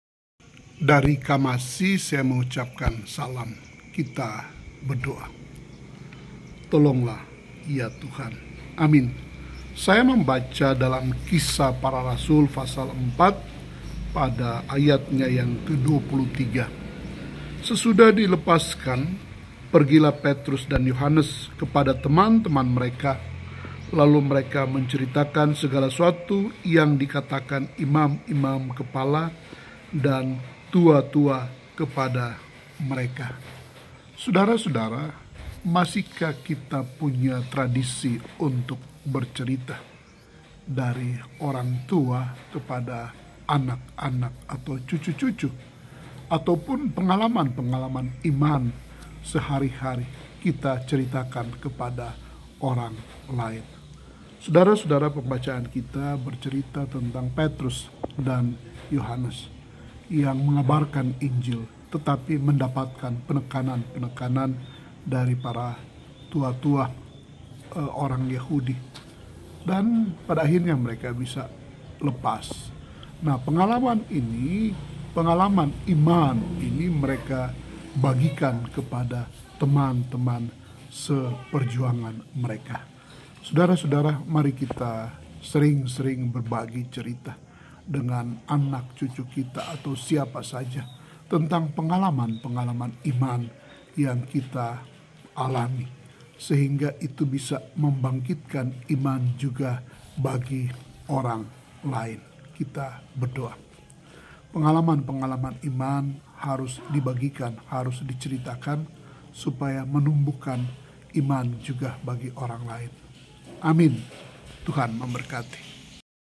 (GPIB Gloria) Renungan Pagi - Selasa 2 Mei 2023
Pdt. Jan Jona LUMANAUW
Sesuai Sabda Bina Umat GPIB